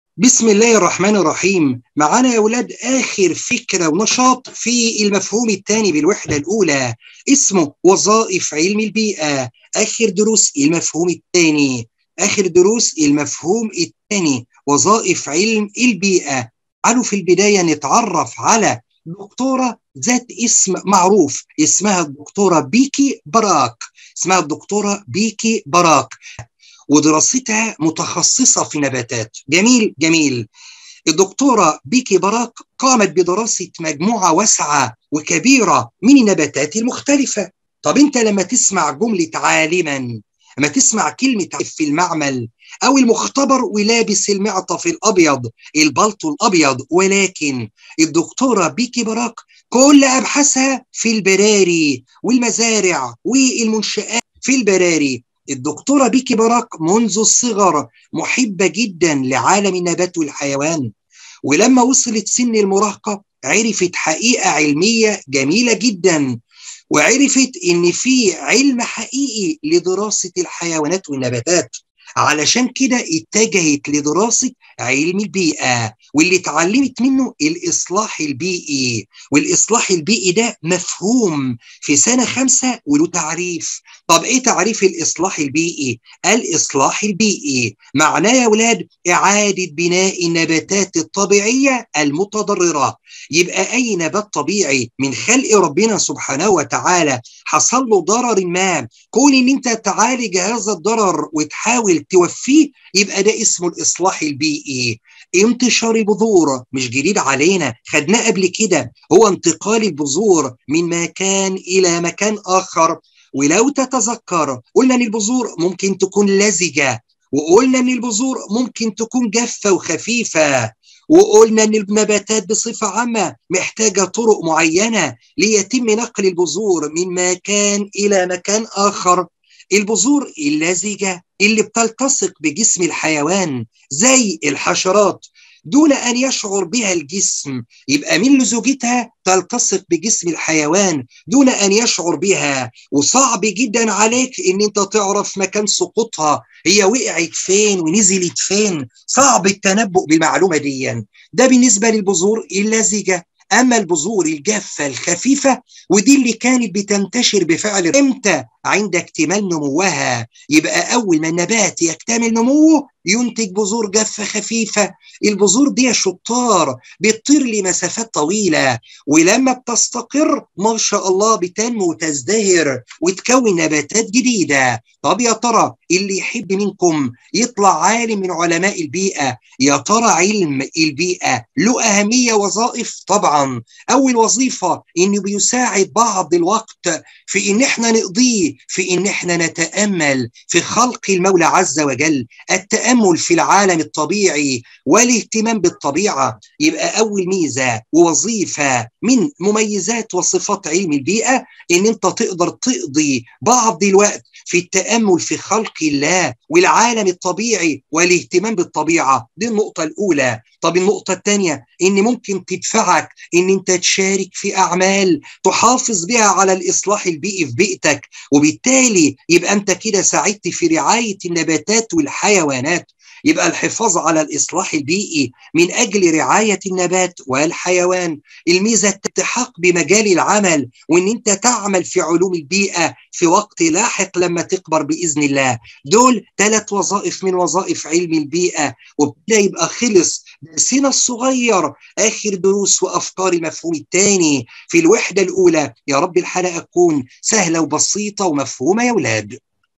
وظائف علم البيئة درس خفيف بسيط من دروس المفهوم الثانى علوم الصف الخامس الجديد مستر حامد البيومى
قناة روائع البيومى
علوم الصف الخامس 2023
 تدريبات وشرح وافي
وظائف علم البيئة